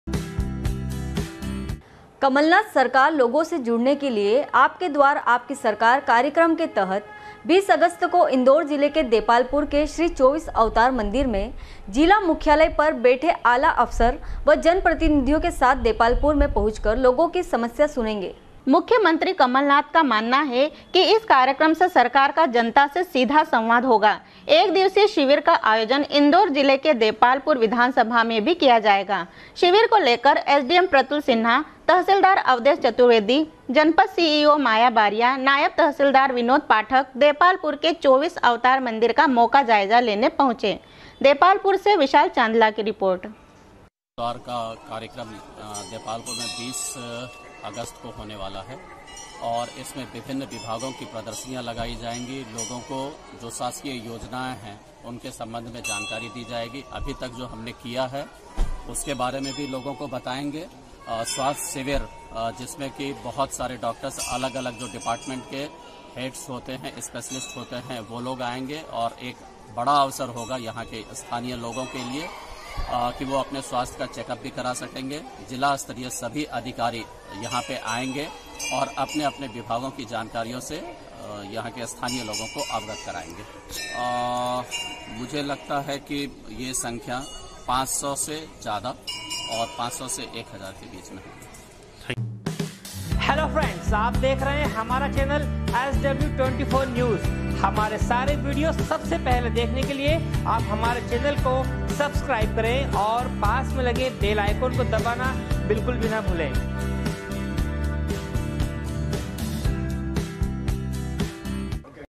sw24news depalpur, 19.08.2019
कमलनाथ सरकार लोगों से जुडने के लिए ‘आपके द्वार, आपकी सरकार’ कार्यक्रम के तहत 20 अगस्त को इंदौर जिले के देपालपुर के श्री 24 अवतार मंदिर में जिला मुख्यालय पर बैठे आला अफसर व जनप्रतिनिधियों के साथ देपालपुर में पहुंचकर लोगों की समस्याएं सुनेंग#sw24news